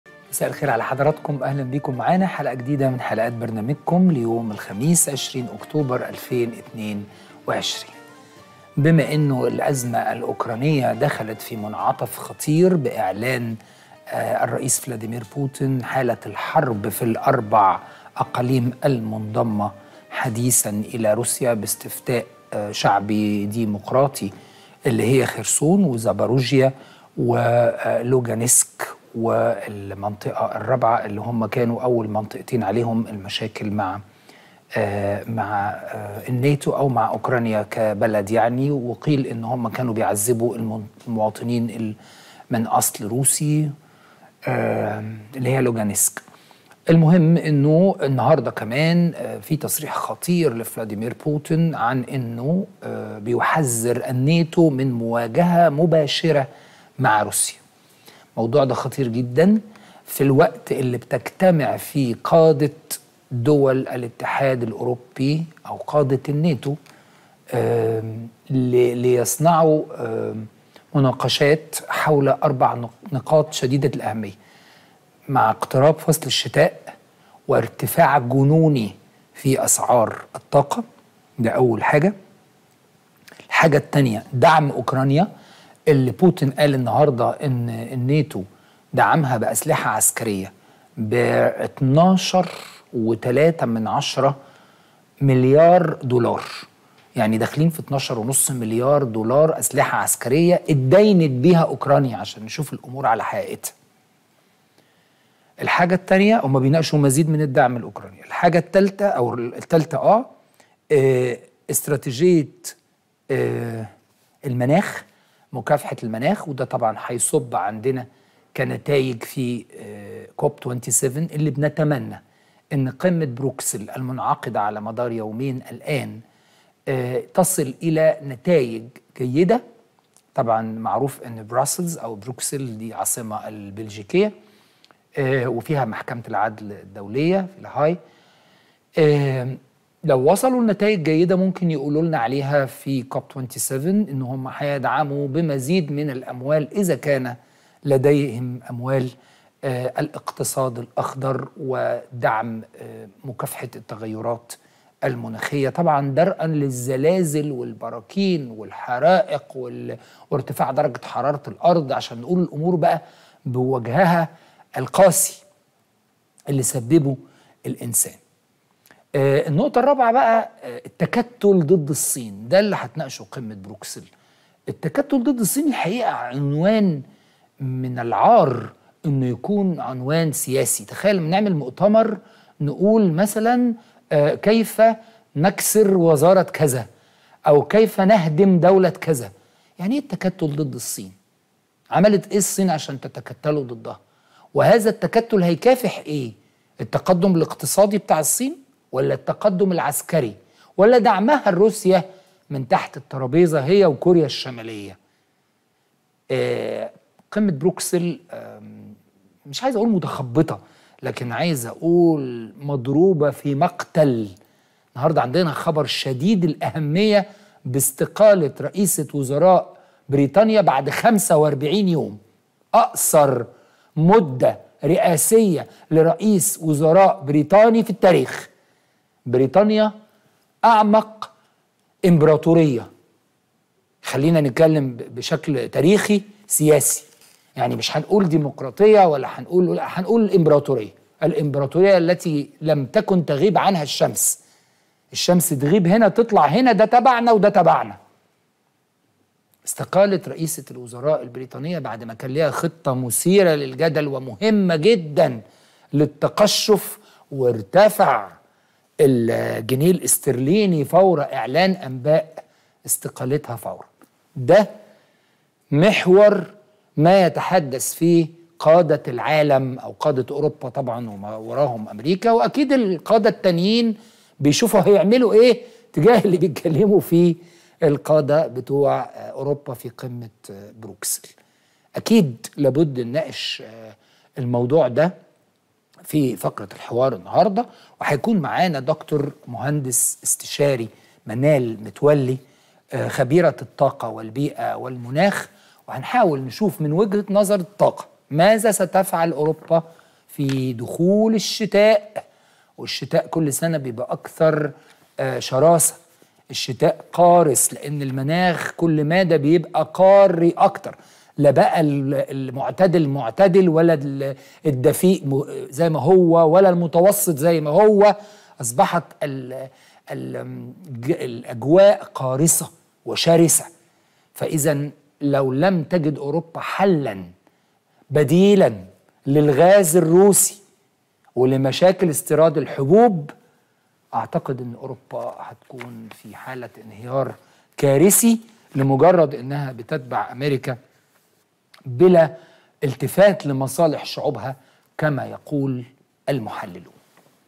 إيهاب صبحي يناقش التكتل ضد الصين تساؤلات وتخبطات ...
إيهاب صبحي يناقش
التكتل ضد الصين تساؤلات وتخبطات ...

على التردد
11603 | ١١٦٠٣
Horizontal | أفقي
27500 | ٢٧٥٠٠
3/4 | ٣/٤